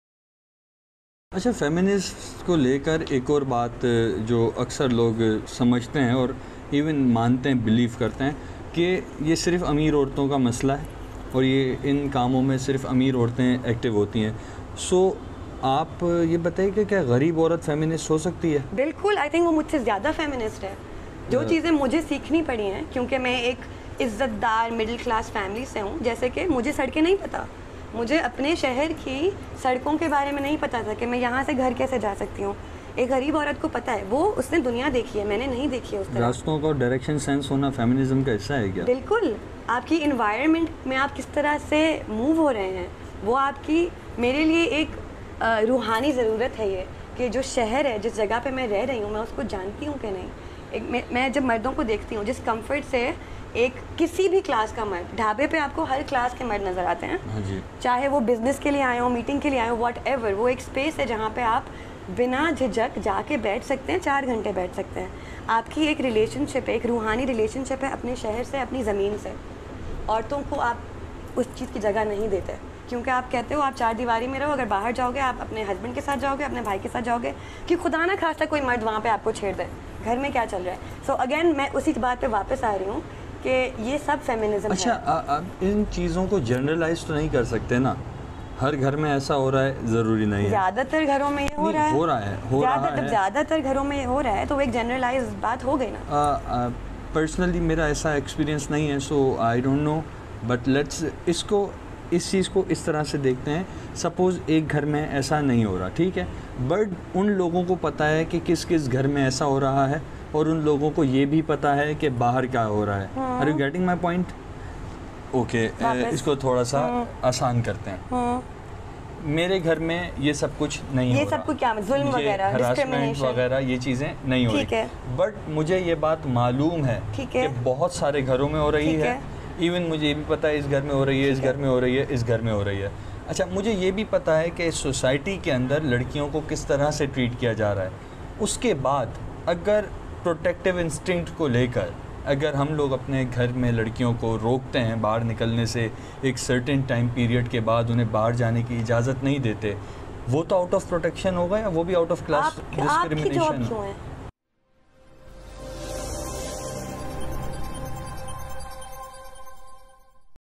Activism and the Elite women ft. Sadia Khatri | Kanwar Naeem
Whether you turn on the television or sit at a restaurant, you will get to hear; politics, aurat march, and all similar activities for the elite women. No moment includes underprivileged women. But this video may change your thoughts.